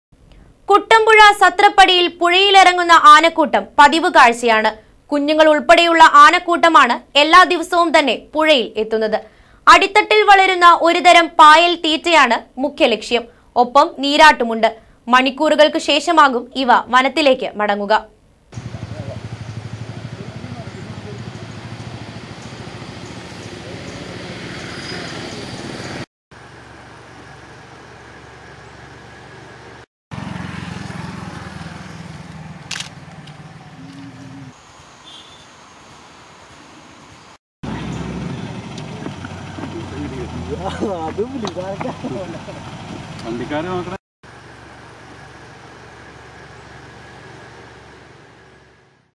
കുട്ടമ്പുഴ സത്രപടിയില്‍ പുഴയിലിറങ്ങുന്ന ആനക്കൂട്ടം
കുട്ടമ്പുഴ സത്രപടിയില്‍ പുഴയിലിറങ്ങുന്ന ആനക്കൂട്ടം പതിവ കാഴ്ചയാണ്.കുഞ്ഞുങ്ങളുള്‍പ്പടെയുള്ള ആനക്കൂട്ടമാണ് എല്ലാദിവസവുംതന്നെ പുഴയില്‍ എത്തുന്നത്.അടിത്തട്ടില്‍ വളരുന്ന ഒരുതരം പായല്‍ തീറ്റയാണ് മുഖ്യലക്ഷ്യം.ഒപ്പം നീരാട്ടുമുണ്ട്.മണിക്കൂറുകള്‍ക്ക ശേഷമാകും ഇവ വനത്തിലേക്ക മടങ്ങുക.